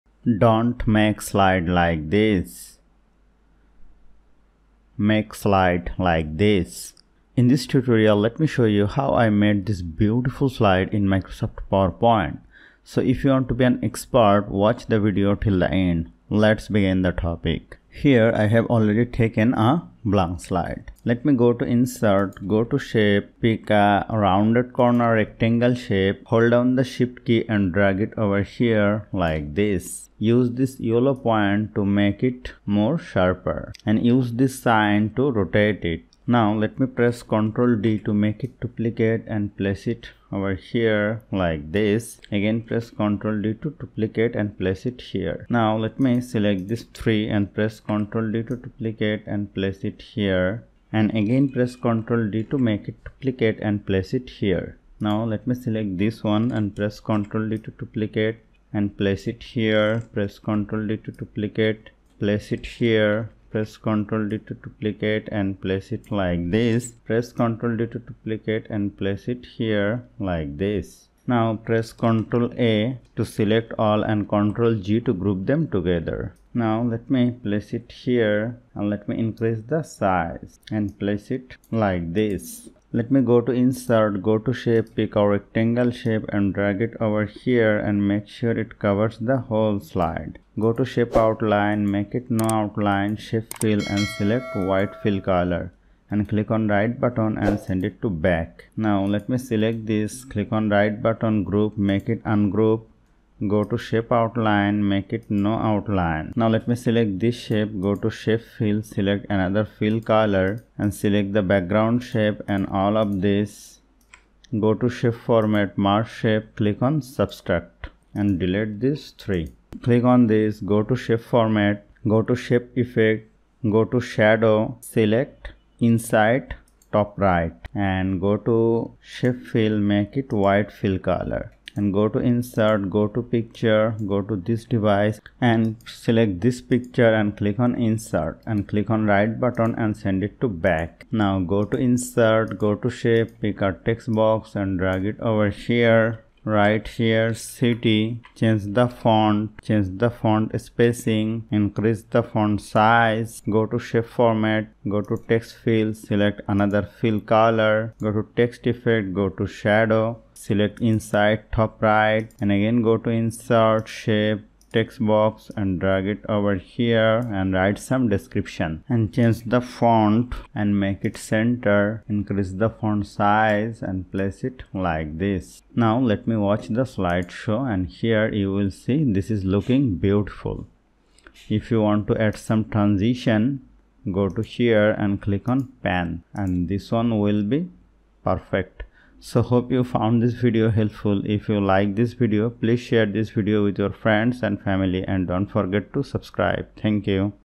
Make Stunning Professional Slide in PowerPoint
Unleash your creativity in PowerPoint with this tutorial on crafting polished and professional slides! Discover the secrets to designing captivating presentations that leave a lasting impact. From layout tips to selecting the perfect fonts and visuals, this video covers it all. No design background required! Join us to learn easy yet effective techniques for creating stunning slides that captivate your audience.